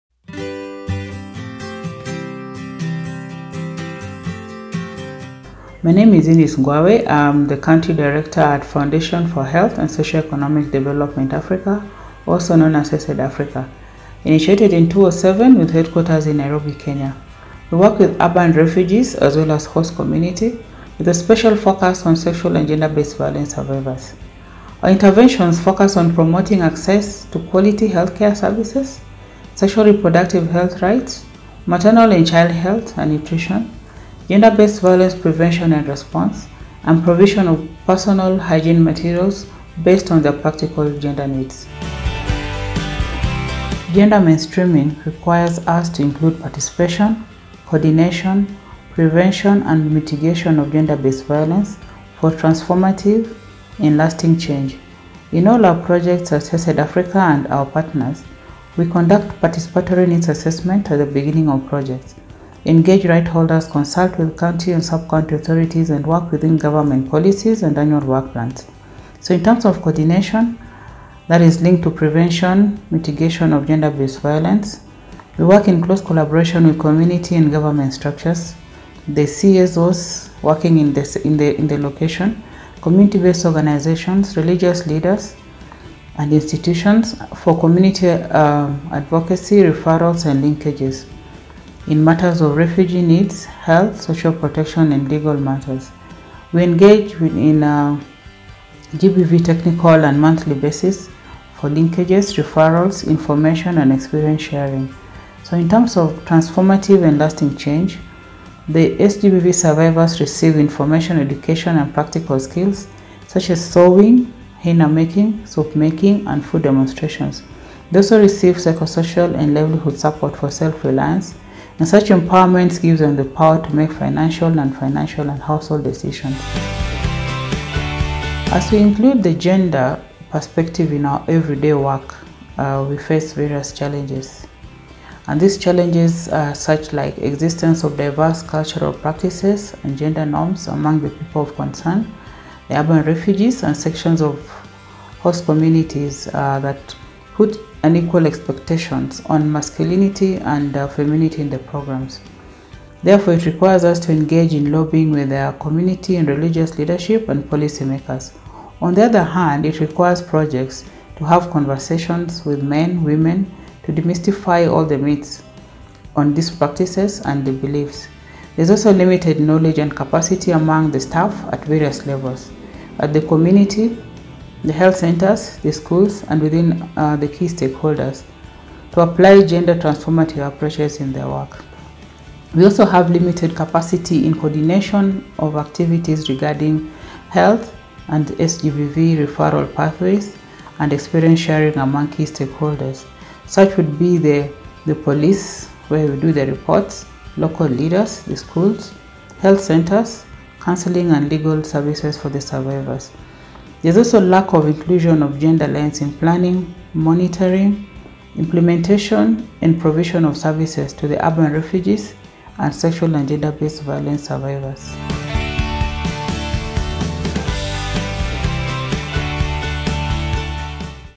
Generoa ekintza humanitarioan. HESED erakundearen esperientzia ( Kenya )
eLankidetzak eta Bilboko Udalak, “Generoa ekintza humanitarioan: nola bihurtu gure konpromisoa errealitate” izenpean Garapenerako Estrategia Positiboen XIII. Jardunaldiak antolatu genituen. Jardunaldiek 2022ko maiatzaren 23an eta 24an bildu zituzten, Bilbon, mundu osoko adituak eta erakundeak ekimen humanitarioetan genero ikuspegia jorratzeko. Afrikako, Latinoamerikako eta Ekialde Hurbileko esperientzia-mahaian haibat herrialdetako esperientziak partekatu ziren.

The  Foundation for Health and Social Economic Development Africa (HESED) erakundeak azaldu zuen Kenyan egin duten lana.

eLankidetza - Lankidetzarako eta Elkartasunerako Euskal Agentzia
eLankidetza - Agencia Vasca de Cooperación y Solidaridad
eLankidetza - Basque Agency for Cooperation and Solidarity
eLankidetza - Agence Basque de Cooperation et Solidarité

Web: elankidetza.euskadi.eus